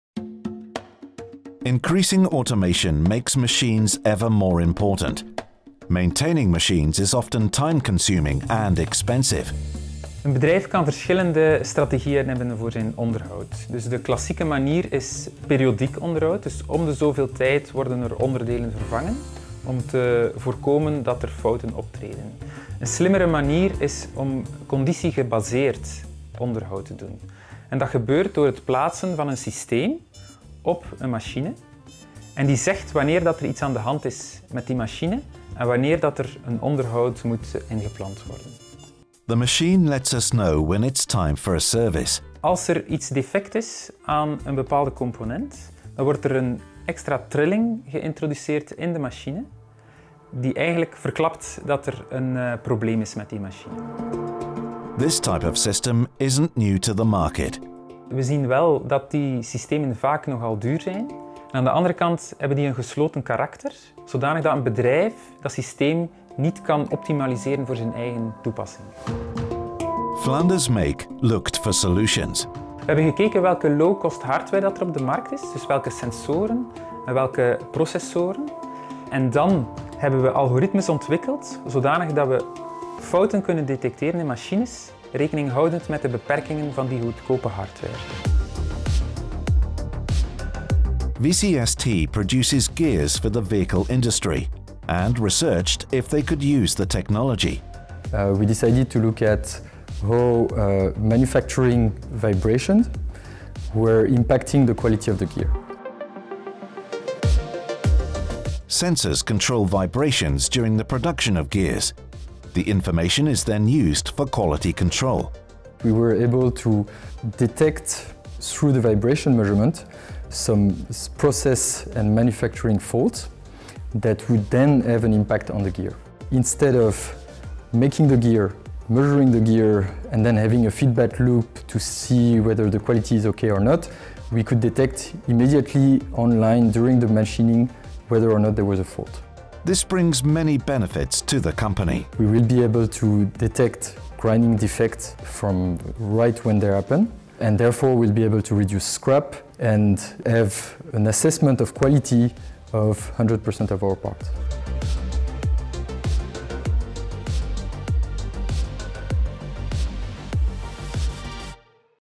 Condition-based monitoring (VIBMON EN)
Increasing automation makes machines increasingly important, but also more complex. Consequently, maintaining these machines is often a time-consuming and costly affair. By switching from periodic maintenance to condition-based maintenance.

To do this, we place sensors on a machine that indicate when maintenance is required.
We also detect machine defects via vibration sensors.

These systems already exist, but we have developed a low-cost solution for this based on off-the-shelf sensors and in-house developed algorithms.